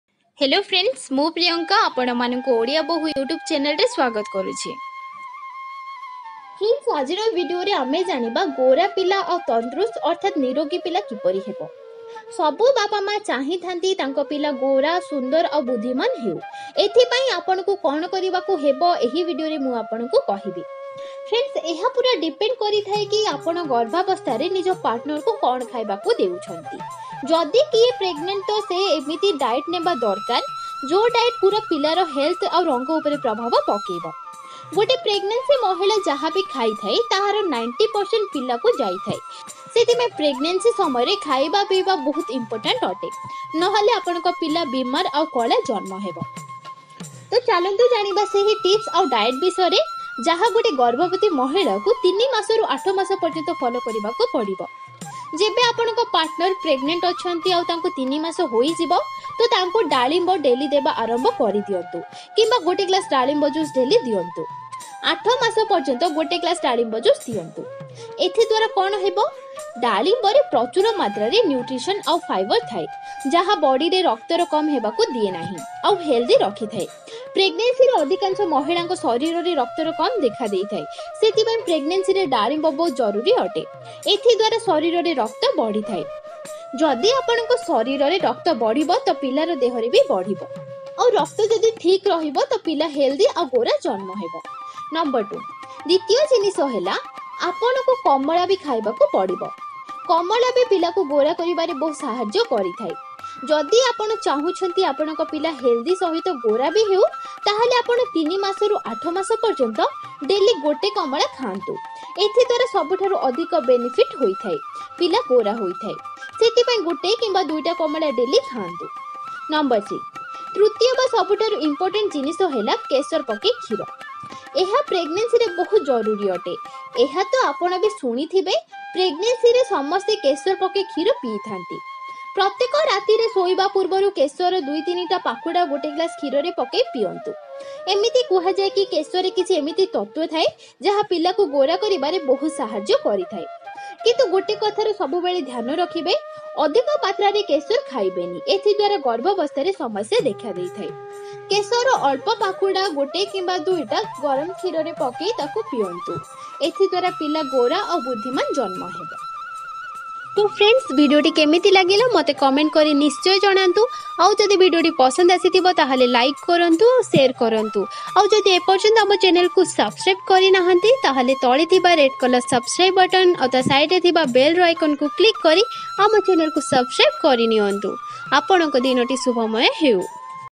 ଗୋରା ଆଉ ବୁଦ୍ଧିମାନ ପିଲା ଜନ୍ମ କରିବାକୁ ହେଲେ ଖାଆନ୍ତୁ ଏହି ଜିନିଷ
Pregnancy samayare ehi jinisa jadi khaibe taahele pila bahut gora aau budhiman janma heba.